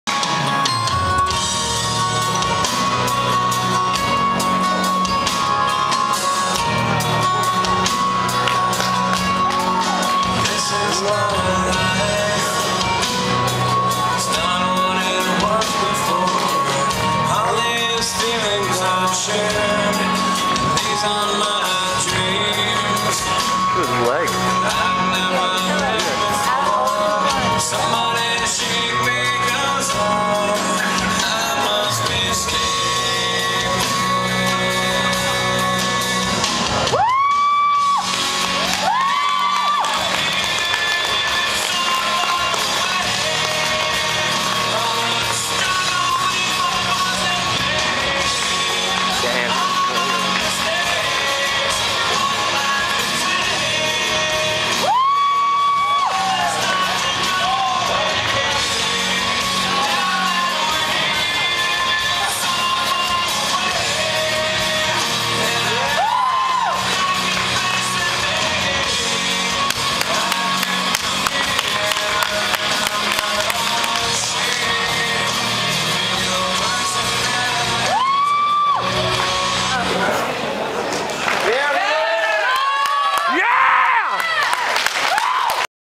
2009 INBF Hercules Posing Routine NY 6-27-09
I placed 3rd in the INBF Hercules Mens Novice Group B.